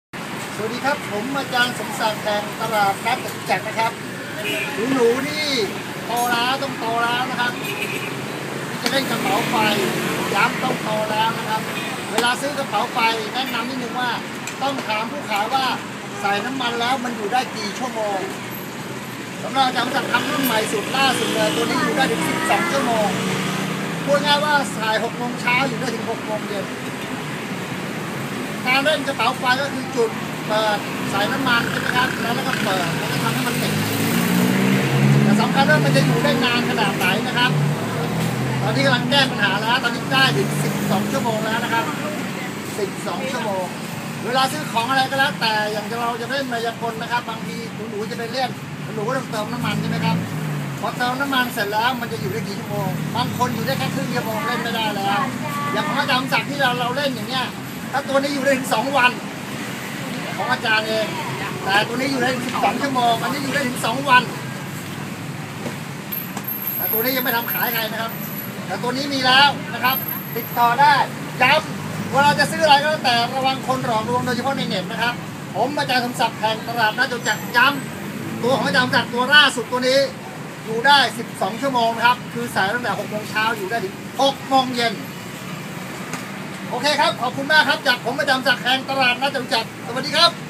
วิธีซื้อกระเป๋าไฟ
วิธีซื้อกระเป๋าไฟ ต้องถามคนขายว่าใส่น้ำมันแล้วอยู่ได้กี่ชั่วโมง รุ่นใหม่ของอ.สมศักดิ์ อยู่ได้12ชั่วโมงครับ